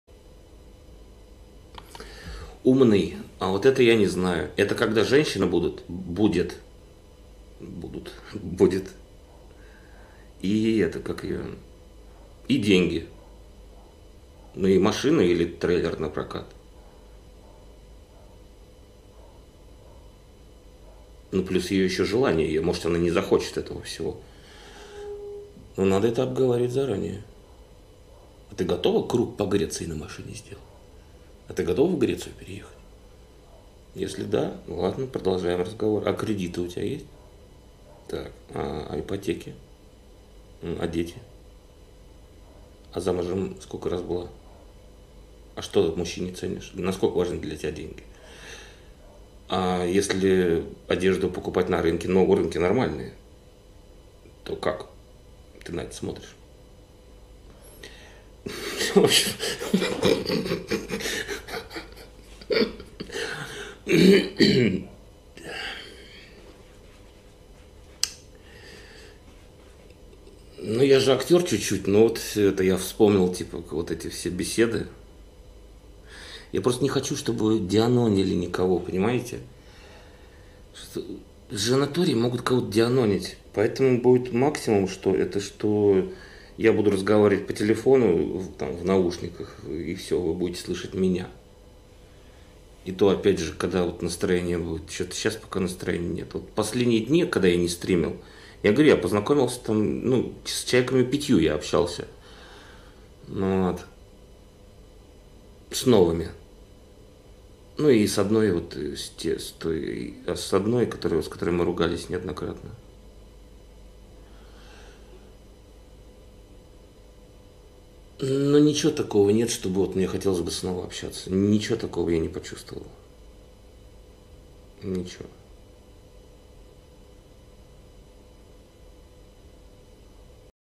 16.10.2022 Влад Савельев. Самые важные вопросы женщине
Владос проговаривает свои традиционные вопросы женщине